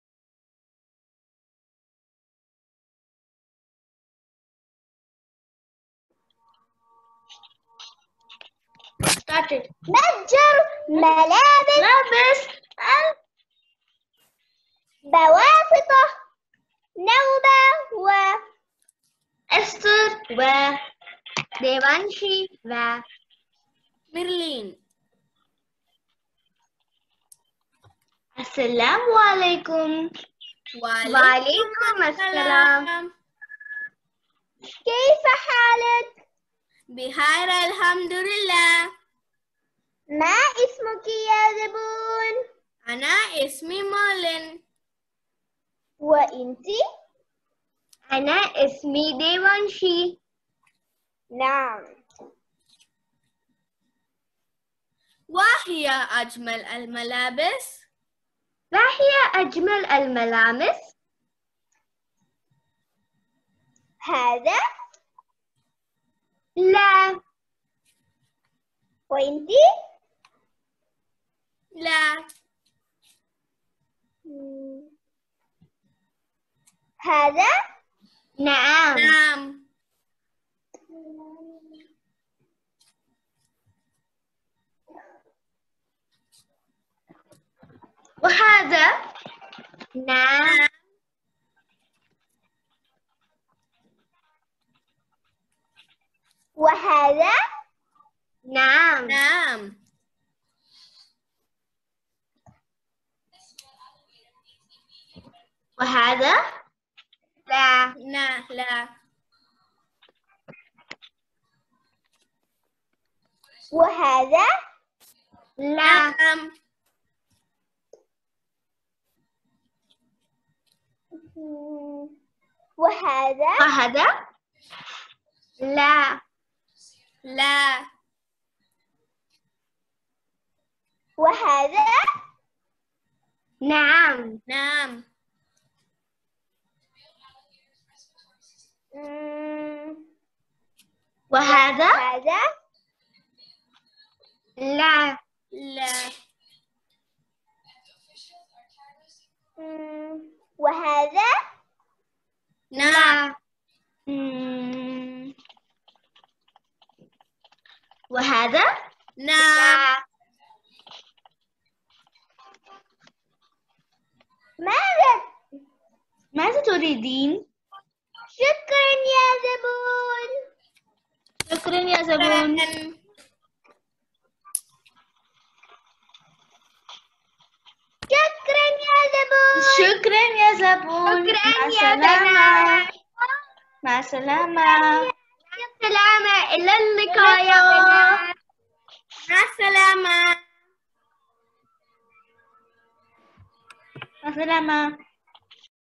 E Learning Activity/Arabic Activity/ cloth shop
This is an Arabic Activity presented by Nauba and her friends Devanshi,Ester and Merlin
E Activity for students
Arabic Activity
E learning activity
Amazing activity
Cloth shop